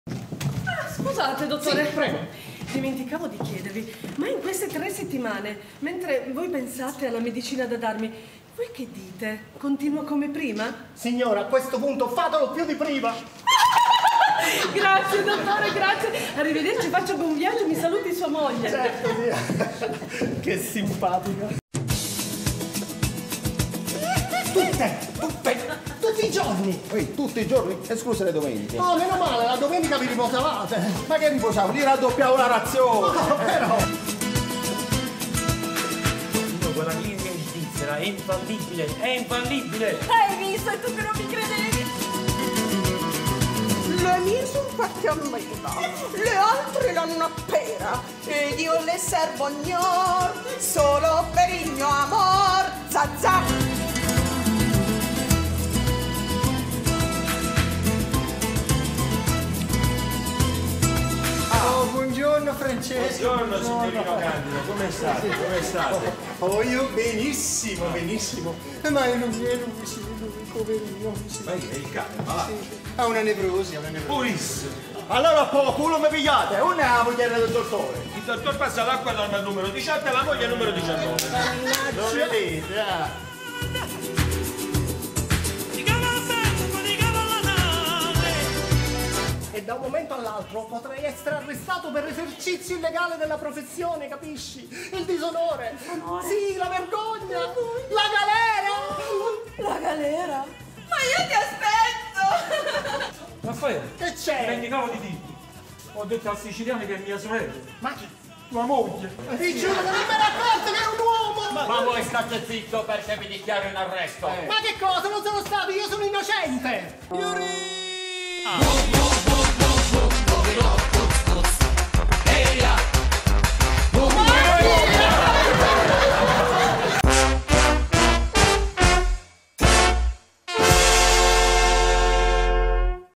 LE PILLOLE DI ERCOLE - Official Trailer | I FIGLI DI ANNA | Commedia Teatrale
Un'anteprima della nostra commedia "Le pillole di Ercole".
Visitate il nostro sito web per controllare le date dei prossimi eventi